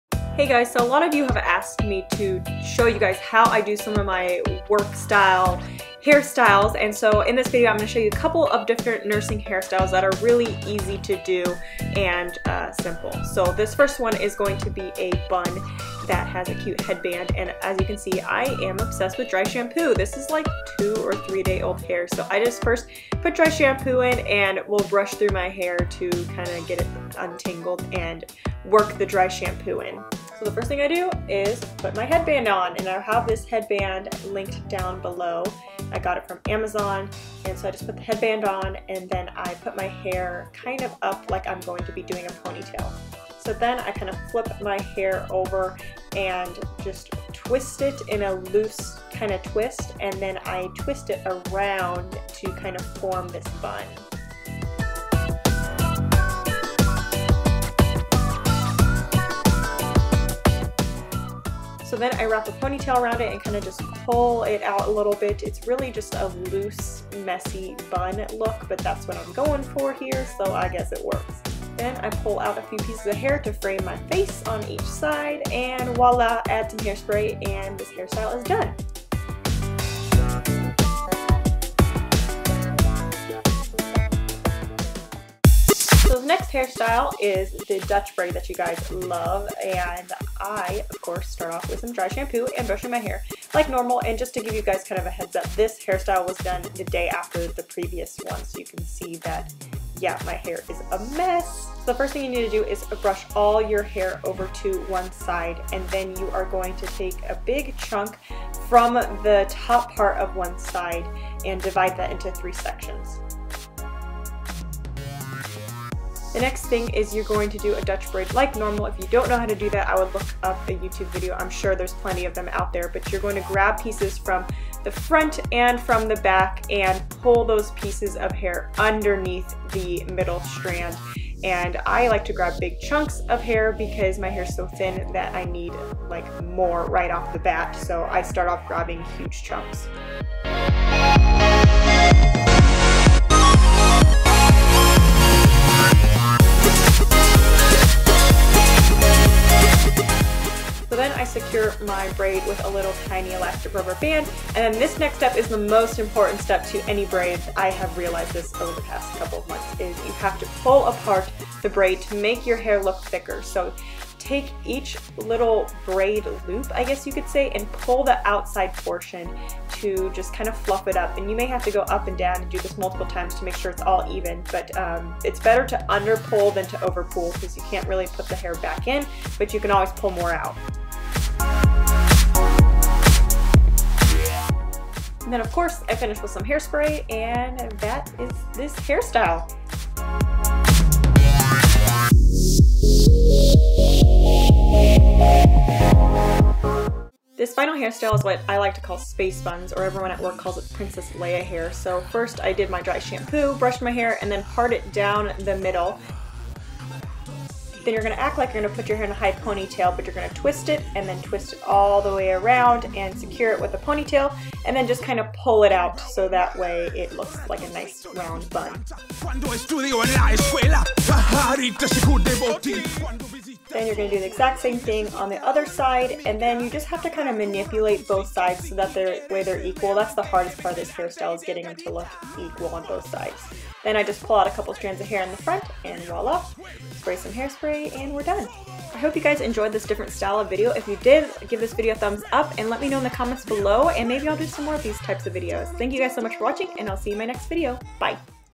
{realistic} NURSING HAIRSTYLES!
Hi, guys! A lot of you ask how I do my hair for my nursing shifts, so I figured I would share a few of my nursing hairstyles that I commonly do! I would love to do more of these types of videos for you all. Also, if you try any of these, make sure you tag me on Instagram (@ashleyadkinsrn) so I can see!!!

LINK TO HEADBANDS

❥Social Sites❥